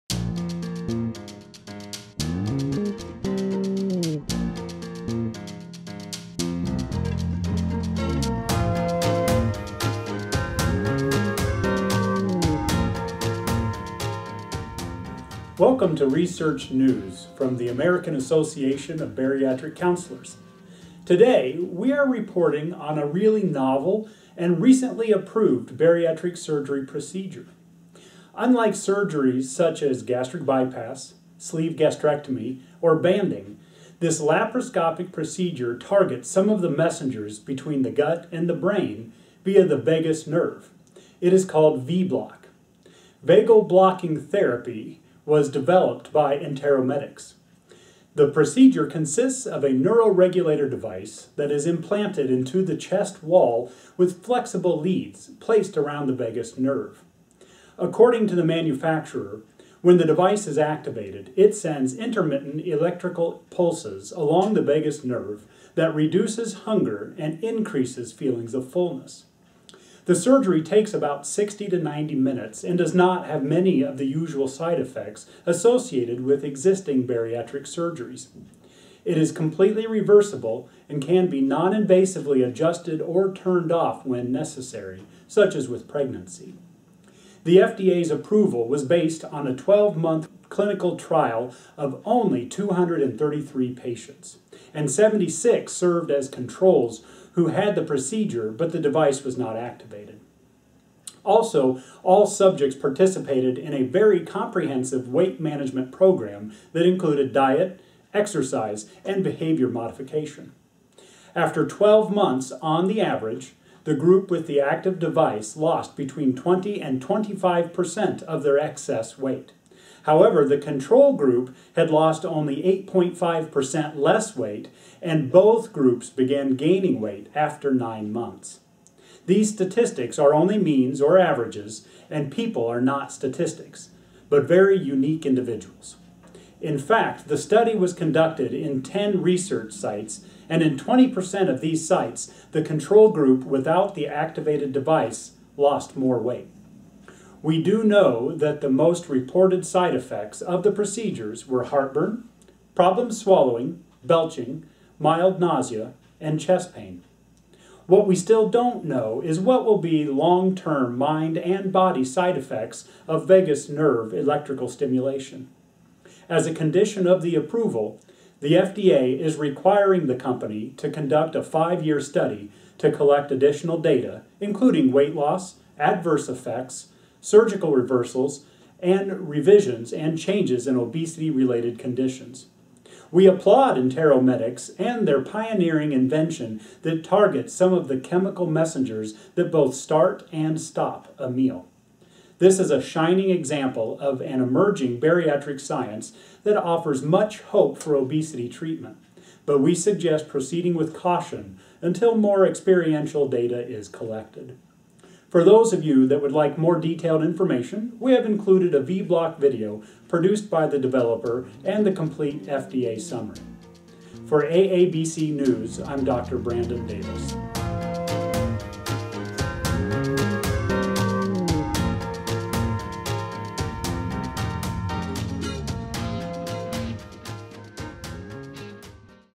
VBloc Bariatric Surgery
Welcome to research news from AABC. Today we are reporting on a  really  novel and recently approved bariatric surgery procedure. Complete FDA Summary
Unlike other surgeries  like  gastric by-pass, sleeves or banding  this laparoscopic  procedure targets some of the messengers between the gut and the brain via the vagus nerve. It is  called VBLOC Vagal Blocking therapy and was developed by EnteroMedics.